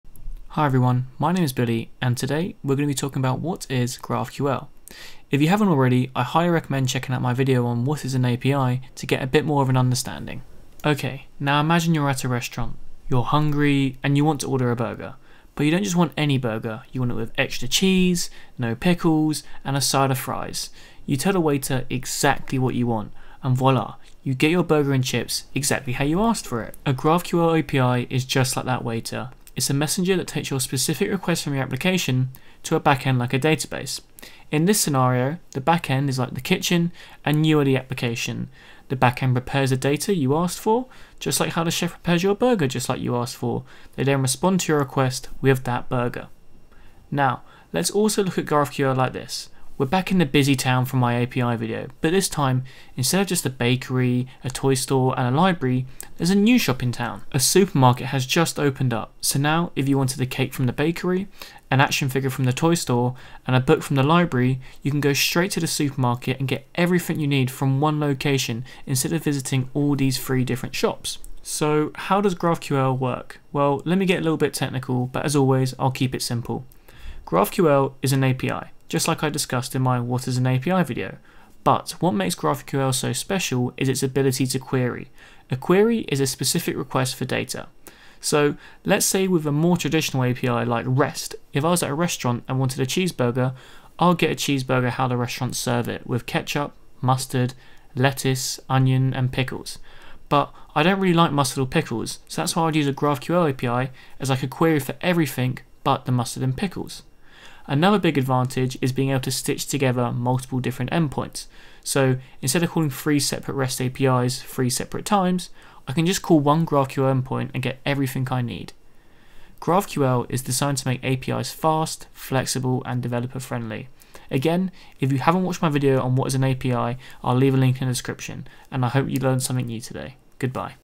What is GraphQL? a very simple explanation
Today we explore what GraphQL is and how it works ! GraphQL is an open-source technology which adds a query language to an API.